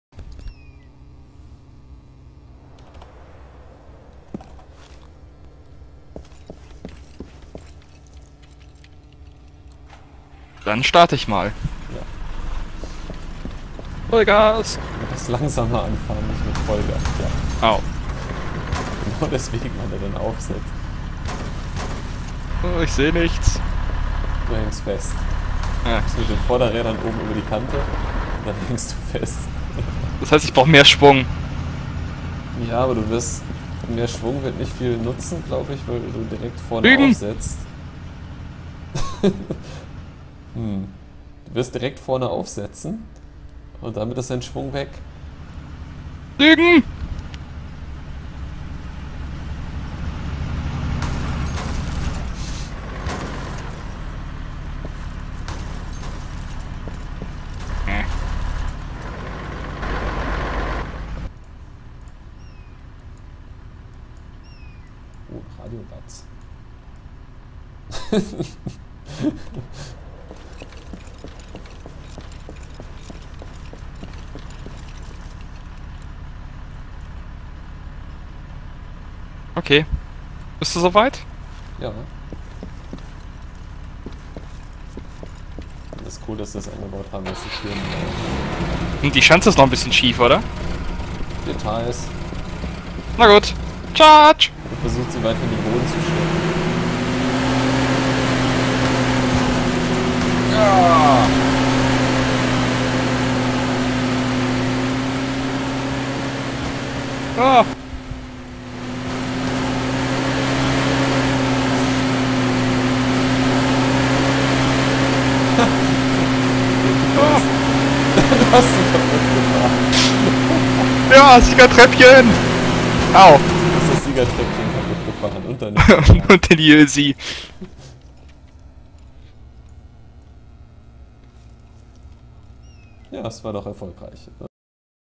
VR Testdrive
So sieht es aus wenn wir produktiv sind und uns ernsthaft daran setzen neue Arrangements für Escape 2 zu bauen...

Nicht im Video zu sehen: Das zwangsweise folgende spontane Deathmatch.